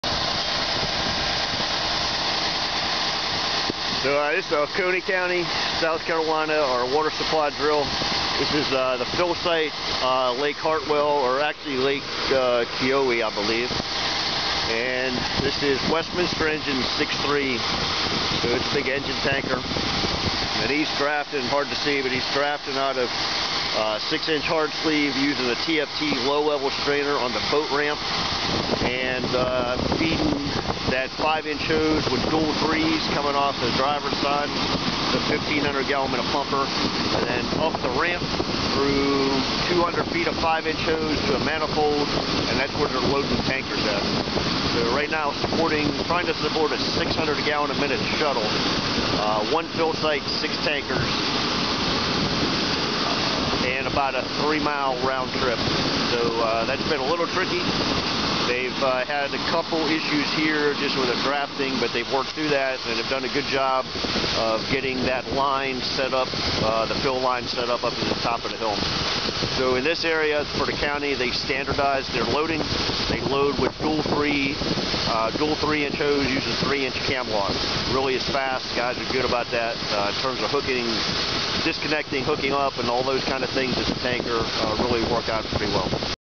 Part 7 - Rural Water Supply Drill - Oconee County, South Carolina - March 2018
In this video clip we take a look at the fill site engine as it drafts from Lake Keowee. Westminster Engine 63 (1500 gpm) was used to draft and load tankers.  The fill site provided a 3-mile roundtrip for rigs hauling water. The clip is narrated by Mark Davis of GBW Associates, LLC.